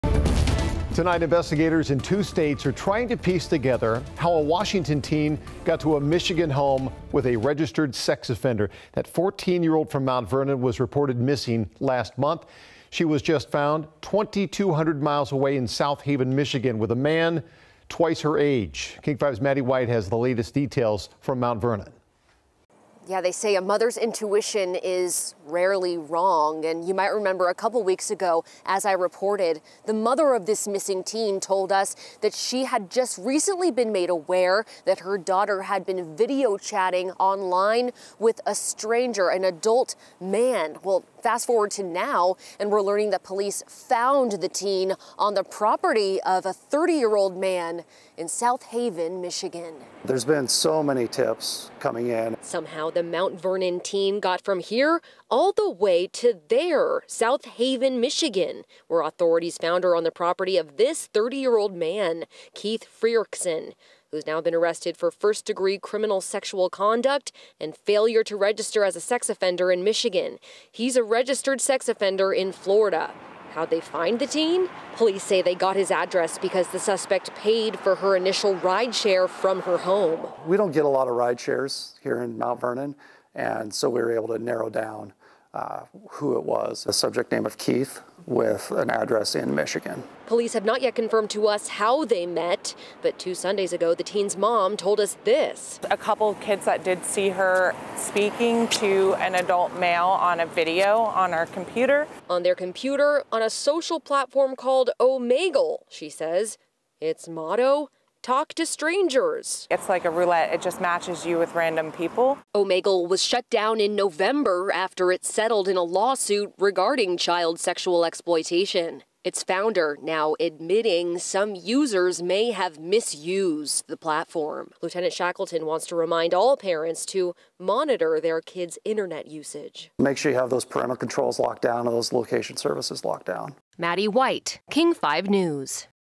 Mount Vernon teen who went missing in early January found in Michigan with registered sex offender
A 14-year-old girl who went missing out of Mount Vernon was found safe in Michigan nearly a month after her initial disappearance, according to the Mount Vernon Police Department (MVPD).

The girl was last seen on Jan. 6. Her friends told the teen's parents that she had been speaking with an older man on the social media website Omegle, and they believed he may be the reason that she left.

As we reported in mid-January, the missing teen's mother told KING 5 she her daughter had been lured by an adult with bad intentions.